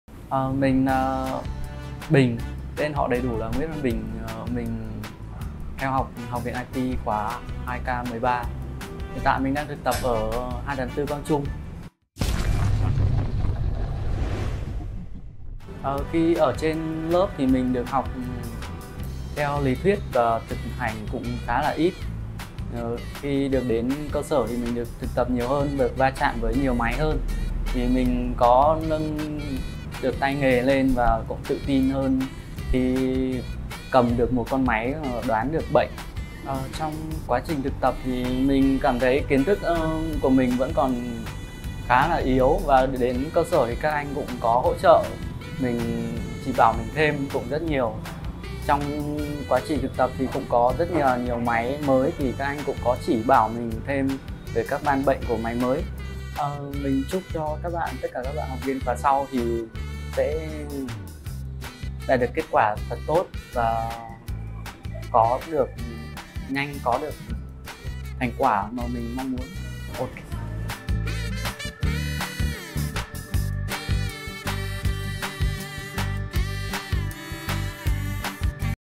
“Thực tập đã giúp mình nâng cao tay nghề và tự tin hơn” - Học viên Bình
Sau 1 tháng học tập tại Học viện iT, Bình đã thành thạo những kiến thức mà các thầy truyền dạy. Thế nhưng, điều đó là không đủ, Bình cần được rèn luyện nhiều hơn và va chạm với đa dạng máy hơn. Và vấn đề này đã được giải quyết nhờ chương trình cho học viên đi thực tập tại các cơ sở, hệ thống sửa chữa laptop uy tín của Học viện iT.
Trong quá trình thực tập, Bình đã được tiếp xúc thêm nhiều dòng máy mới cũng như các pan bệnh lạ trên laptop - Đây là điều Bình không thể có được khi chỉ học trên lớp. Nhờ đó, Bình nhận ra được những kiến thức, kỹ năng còn yếu của mình để trau dồi thêm và hoàn thiện bản thân.

Thực hiện
Nội dung: Tâm Nemo
Quay phim | Hậu kỳ: Emiya Khoa
Dẫn | Reviewer:

Thông tin :
🔶 Cập nhật thông tin khóa học mới nhất tại:

© Bản quyền thuộc về Học viện IT.vn